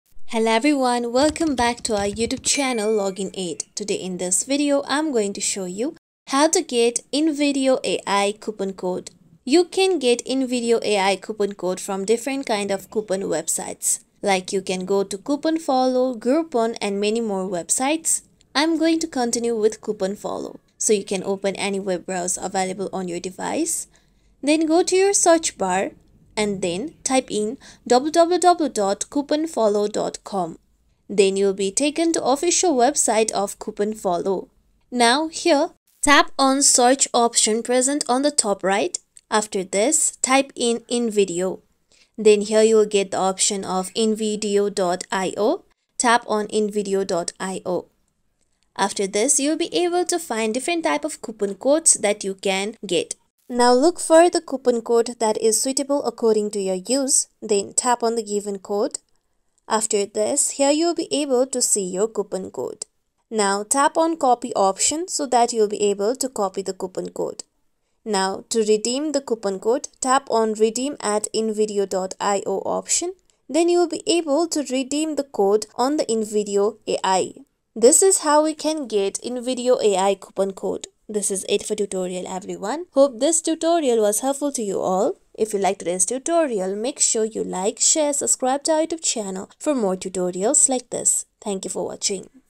How To Get InVideo AI Coupon Code 2024 | Find & Apply InVideo Coupon Code
Discover how to get an InVideo AI coupon code with this easy-to-follow guide. We'll walk you through the steps to find and apply coupon codes to save on its subscriptions and services. Perfect for users looking to unlock discounts and deals on InVideo AI.